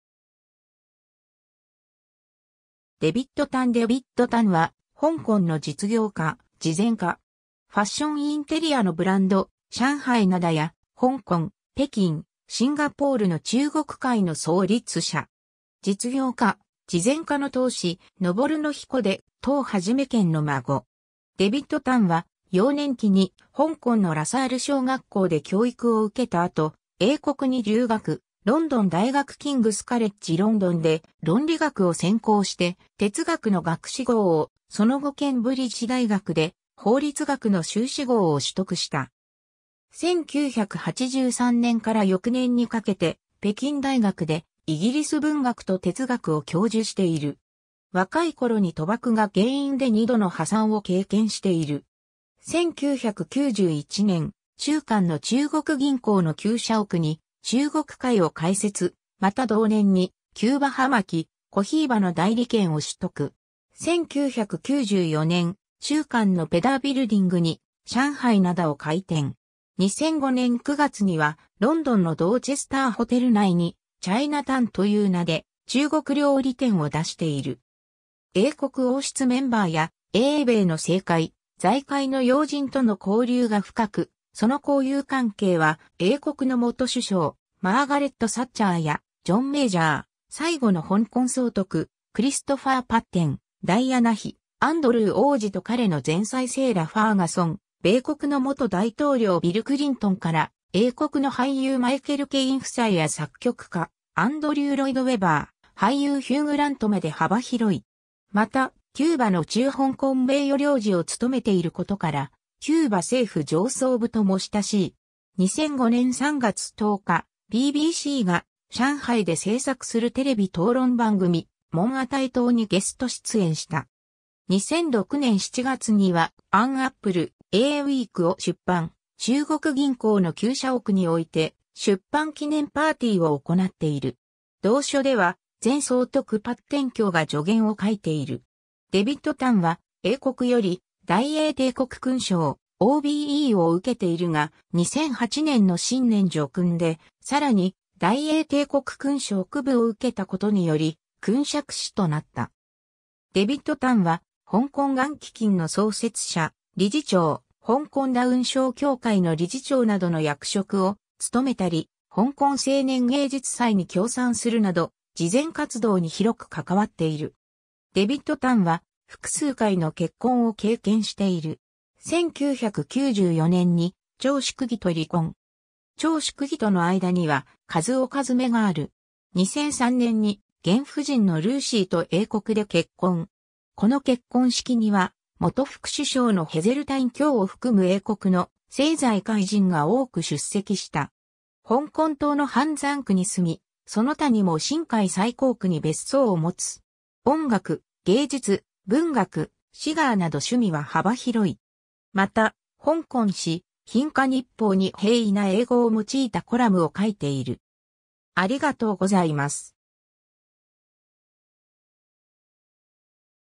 デヴィッド・タン
キングス・カレッジ・ロンドン出身の人物
デヴィッド・タン（2014年） デヴィッド・タン（Sir David Tang Wing-cheung KBE、鄧永鏘、1954年8月2日 - 2017年8月29日）は、香港の実業家・慈善家。
ファッション・インテリアのブランド上海灘（シャンハイ・タン）や、香港・北京・シンガポールの中国会（チャイナ・クラブ）（zh）の創立者。
実業家・慈善家の鄧志昂の曾孫で、鄧肇堅の孫。
 デヴィッド・タンは、幼年期に香港のラ・サール小学校 （zh）で教育を受けた後、英国に留学、ロンドン大学キングス・カレッジ・ロンドンで論理学を専攻して哲学の学士号を、その後ケンブリッジ大学で法律学の修士号を取得した。
1983年から翌年にかけて北京大学でイギリス文学と哲学を教授している。
 若い頃に賭博が原因で2度の破産を経験している。
1991年、中環の中国銀行の旧社屋（zh）に中国会を開設、また同年にキューバ葉巻「Cohiba」の代理権を取得。
1994年、中環のペダー・ビルディング（zh）に上海灘を開店。
2005年9月にはロンドンのドーチェスター・ホテル内に「China Tang（唐人館）」という名で中国料理店を出している。
 英国王室メンバーや英米の政界・財界の要人との交流が深く、その交友関係は英国の元首相マーガレット・サッチャーやジョン・メージャー、最後の香港総督クリストファー・パッテン、ダイアナ妃、アンドルー王子と彼の前妻セーラ・ファーガソン、米国の元大統領ビル・クリントンから、英国の俳優マイケル・ケイン夫妻や作曲家アンドリュー・ロイド＝ウェバー、俳優ヒュー・グラントまで幅広い。
また、キューバの駐香港名誉領事を務めていることから、キューバ政府上層部とも親しい。
 2005年3月10日、BBCが上海で製作するテレビ討論番組「問與答」にゲスト出演した。
2006年7月には『An Apple a Week』を出版、中国銀行の旧社屋において出版記念パーティーを行っている。
同書では前総督パッテン卿が序言を書いている。
 デヴィッド・タンは、英国より大英帝国勲章OBEを受けているが、2008年の新年叙勲でさらに大英帝国勲章KBEを受けたことにより勲爵士となった。
 デヴィッド・タンは、香港がん基金の創設者・理事長、香港ダウン症協会の理事長などの役職を務めたり、香港青年芸術祭に協賛するなど慈善活動に広く関わっている。
 デヴィッド・タンは複数回の結婚を経験している。
1994年に張淑儀と離婚。
張淑儀との間には一男一女がある。
2003年に現夫人のルーシー（Lucy Wastnage）と英国で結婚。
この結婚式には元副首相のヘゼルタイン卿（en）を含む英国の政財界人が多く出席した。
 香港島の半山区に住み、その他にも新界西貢区に別荘を持つ。
音楽、芸術、文学、シガーなど趣味は幅広い。
また、香港紙「蘋果日報 (香港)」に平易な英語を用いたコラムを書いている。